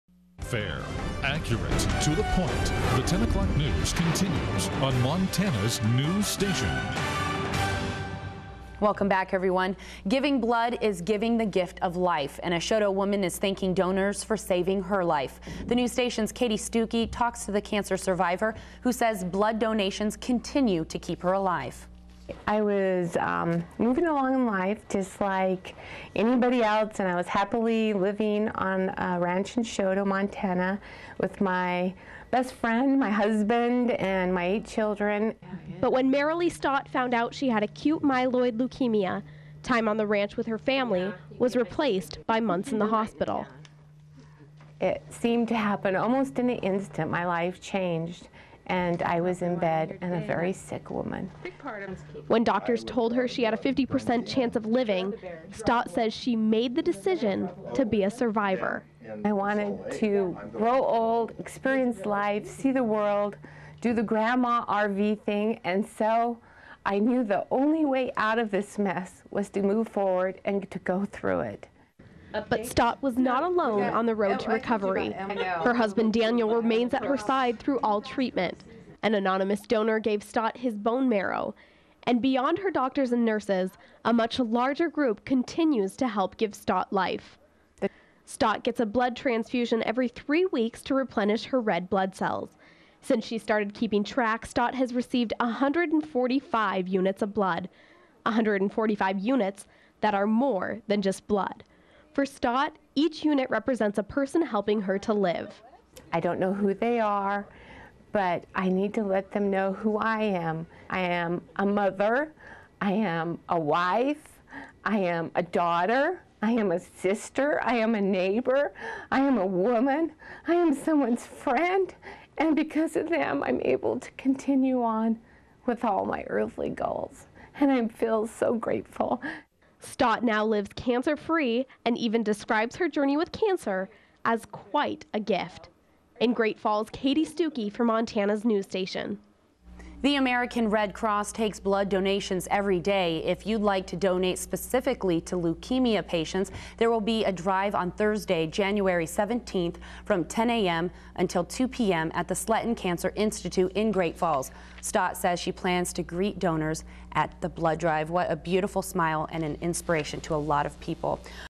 Marilee Mitchell Stott Leukemia and importance of Blood Nov 2008
This was a news report on our friend Marilee Mitchell Stott.  It was reported in November of 2008.  Some will remember her or her father Duke Mitchell.  This is a wonderful story that tells the importance of blood to many people.  Although my wife Jenny Erickson is going through Leukemia treatment right now hers is a subset of the leukemia that Marilee had.  It is October 14, 2019 and Jenny has been getting daily treatment (chemo, infusion and oral).  She has 7 more months of treatment and we pray the outcome will be the same as Marilee's even though it is probable that Jenny will not have to have a bone marrow transplant.  We do seek your prayers.